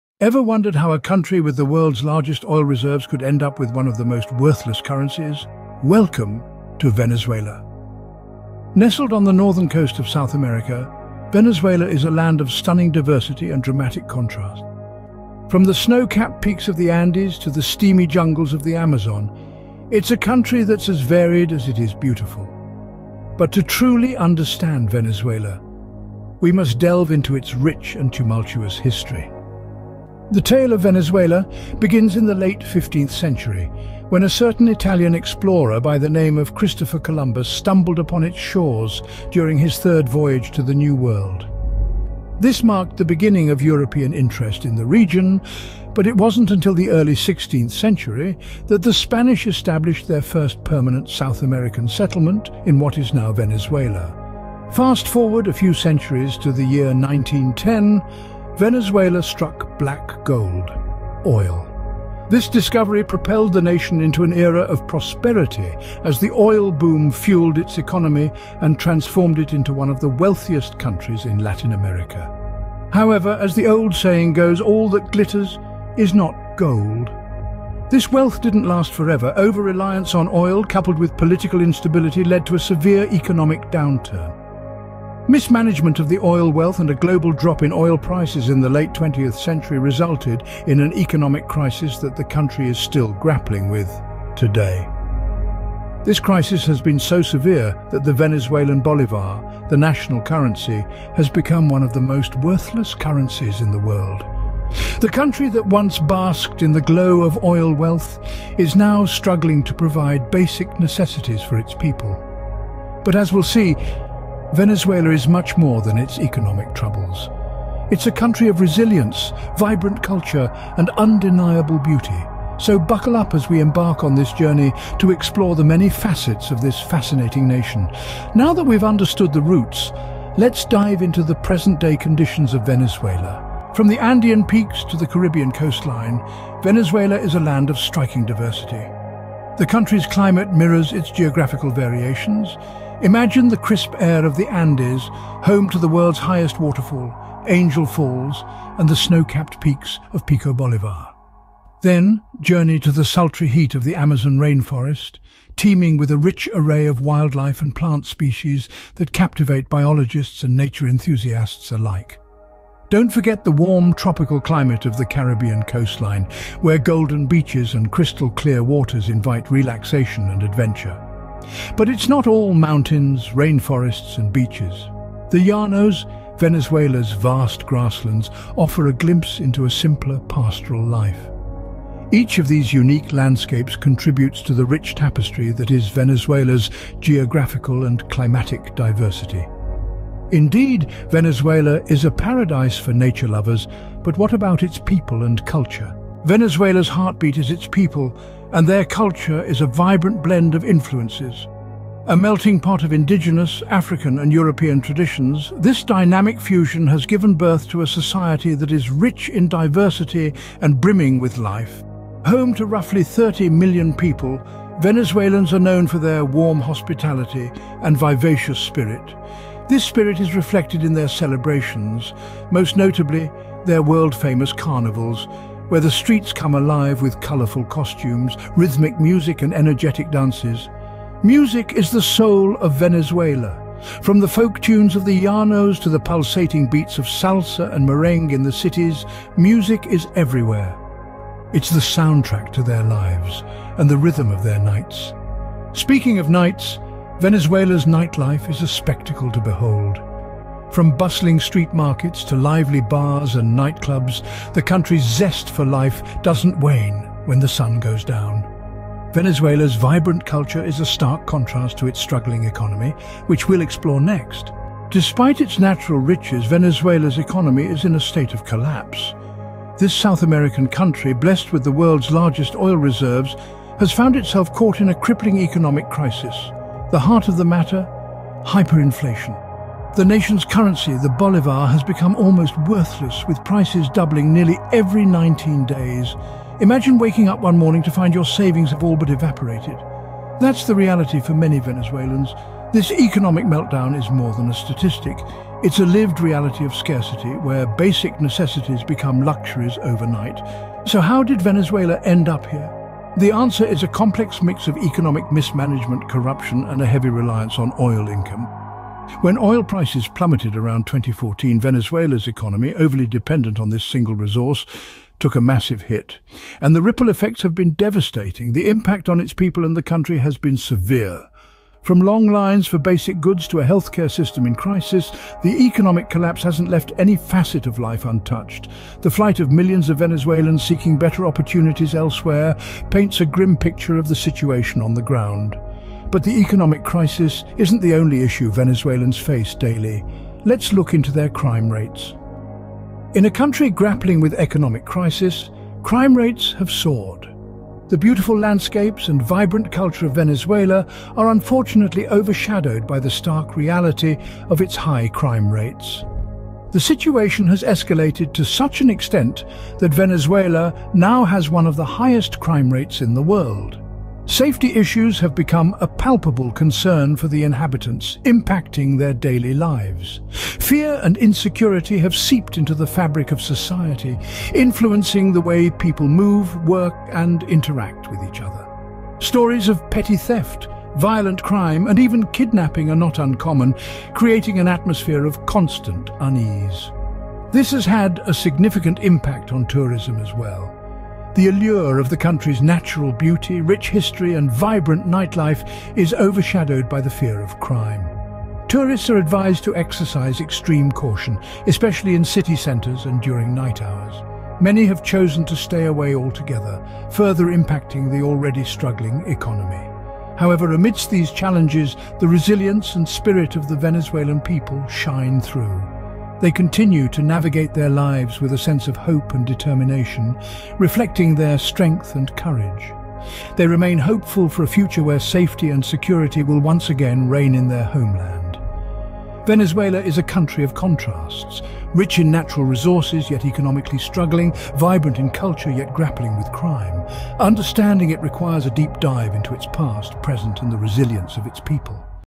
Venezuela: Economic Challenges and Cultural Riches and Vibrancy
Embark on a riveting exploration of Venezuela, where economic challenges, cultural intricacies, and vibrant dynamism intersect. This documentary unravels the intricate tapestry of Venezuela's economic landscape, shedding light on its resilience amid adversity. From the historical echoes of the Bolivar era to contemporary nuances, witness the nation's diverse heritage manifested in its art, music, and traditions.

Navigate the urban pulse of cities like Caracas and Maracaibo, immersing yourself in the rhythmic beats of daily life from sunrise to sunset. Amidst economic struggles, discover the unwavering spirit that defines Venezuela. Traverse the natural wonders, from the awe-inspiring landscapes of Canaima National Park to the majestic cascades of Angel Falls.

This documentary encapsulates the dichotomy of day and night, capturing the essence of Venezuela's character. It's a visual journey through a land defined by contrasts, resilient spirit, and unyielding vibrancy — offering a comprehensive glimpse into Venezuela's unique identity.
Resilient Culture
Dynamic Urban Life
Economic Resilience
Rich Cultural Heritage
Colorful Traditions
Social Resilience
Historical Echoes
Artistic Flourish
Urban Pulse
Unyielding Spirit

00:00 Introduction and Brief History
02:13 Climate and Geography
03:17 People, Culture and Nightlife
04:26 Economic State and Currency
05:41 Crime Rates